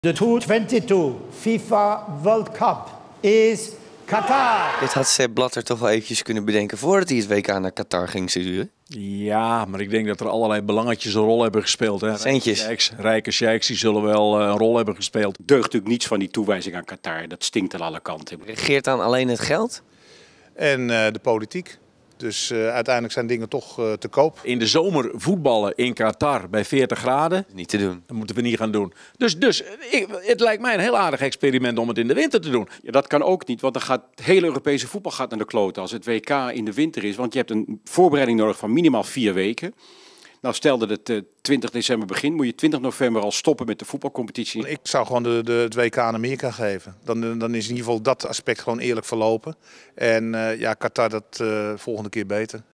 WK-voetbal in 40 graden
Het WK van 2022 wordt in Qatar gehouden. In de zomer. Als het daar zo'n 40 graden is.